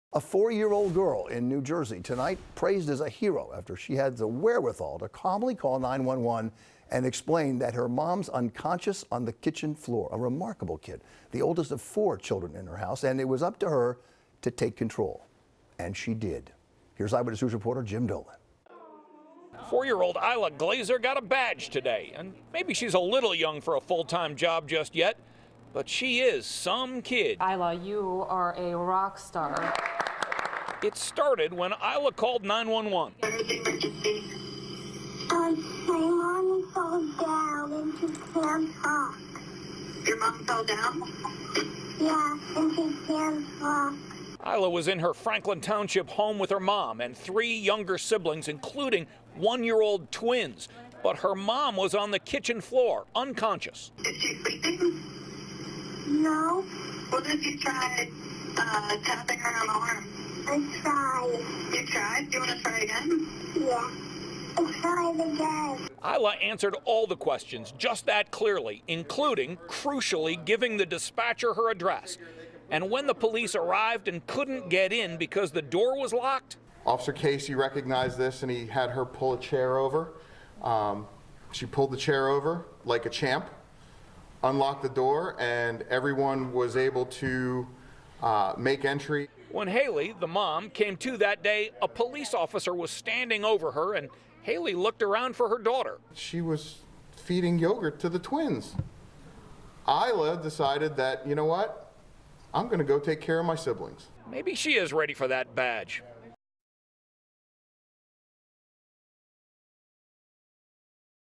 4-year-old hailed as hero for calling 911 and saving mom
A 4-year-old girl in New Jersey is being praised as a hero after she had the poise to call 911 and save her mother's life.

Dispatchers in Franklin Township received the call from a home on Bartle Road shortly before 8 a.m. December 6.

They learned that the caller was 4-year-old Isla Glaser, who said that her mom had fallen down and needed help.

Dispatcher: "What is the emergency?"
Isla:  "My mom fell down and she can't talk."
Dispatcher: "Your mom fell down".
Isla: "Yeah, and she can't talk."

Isla was in her home with her mother, Haley Glaser, and three younger siblings, including 1-year-old twins.

But her mom was on the kitchen floor, unconscious.

Dispatcher: "Is she sleeping?"
Isla:  "No".
Dispatcher:  "Did you try tapping her arm?"
Isla: "I tried."
Dispatcher: "Do you want to try again?"
Isla:  "I tried again."

She was able to provide all the information needed to get help to the house quickly, police said.

NEW HERE? –